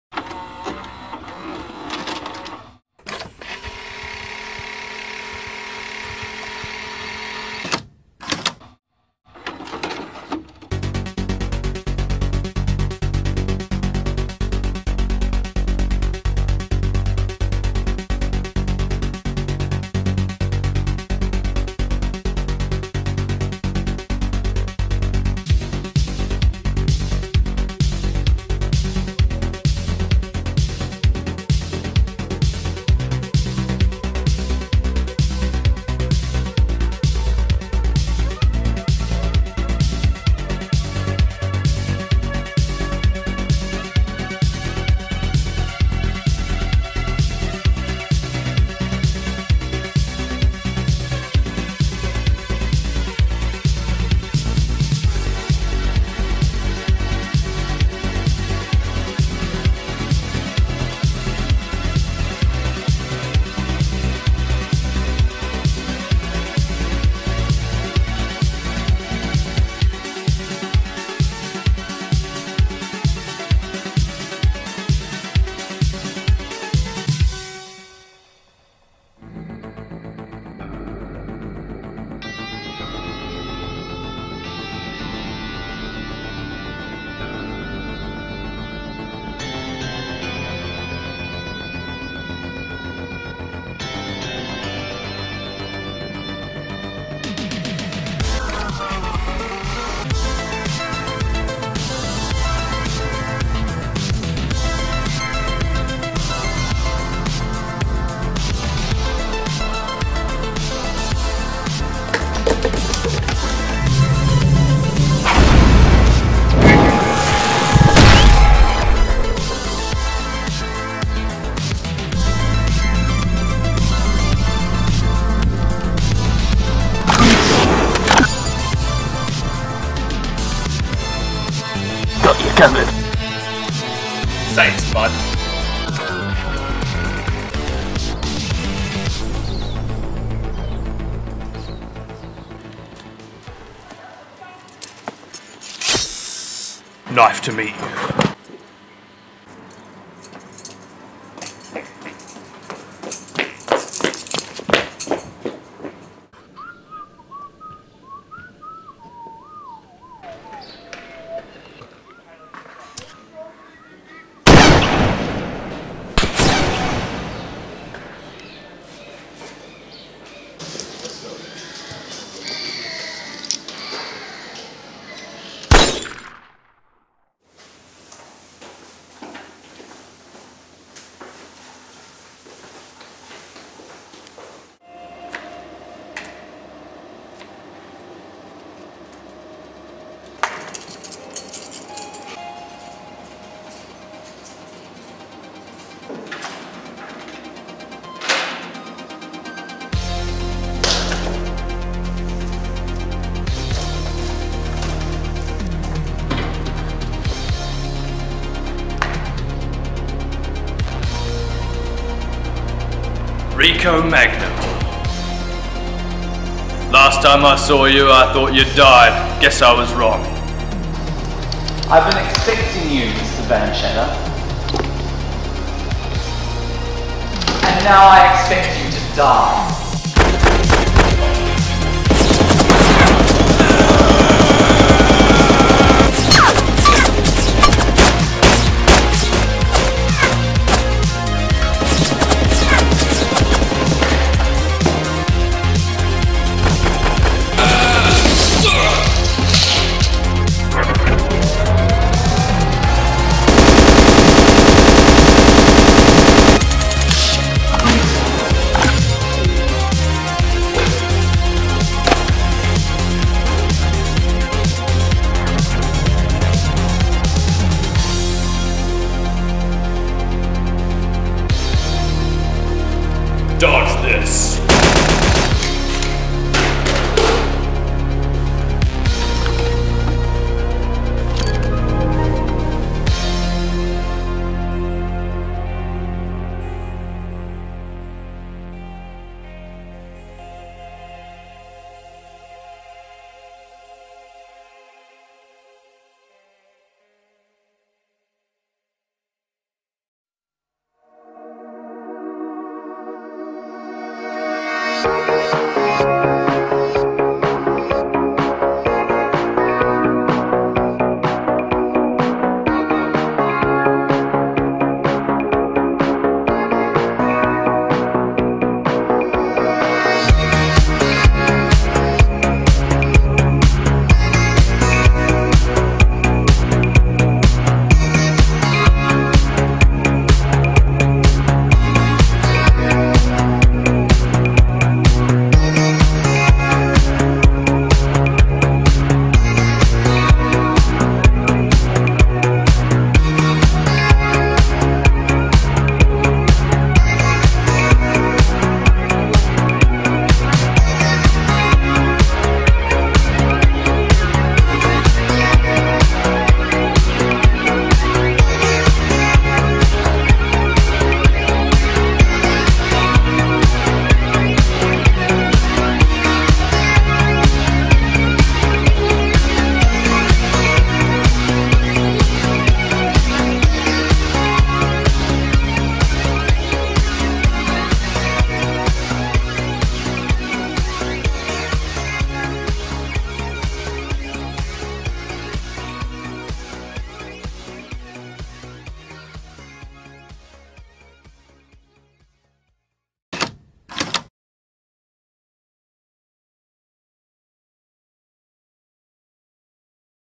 Terminal Action - Extreme Vengeance (2017 Remaster)
A Remastered Version of a student film created in 2015.

TERMINAL ACTION

PLOT

The year is 2007. In the City of Duntropolis, a man named Rico Magnum has taken over the abandoned factory: Hogan Lark Industries, to mass produce his cyborg army. Secret Agent Rock Vanshatter has been sent in to take him out. But he's not the only one. With the help of his sidekick, Dirk Cheapshot, They must infiltrate the base and stop the production.

*Harrison Kennedy (Rock Vanshatter)
*Nik Nazme (Guard #1)
*Zak Booth (Guard #2)
* Nic Lim (Dirk Cheapshot)
* Guy Russell (Guard #3)
* Rani Cohen (Guard #4)
* Aidan Fisher (Guard #5)
* Caide Fallowfield (Guard #6)
* Jack Carter (Guard #7)
* William Kilgour-Hand (Guard #8)
*Michael Lagocki (Rico Magnum)

Special thanks to the amazing Carpenter Brut, Tommy 86, Scattle, Lifelike and Lazerhawk!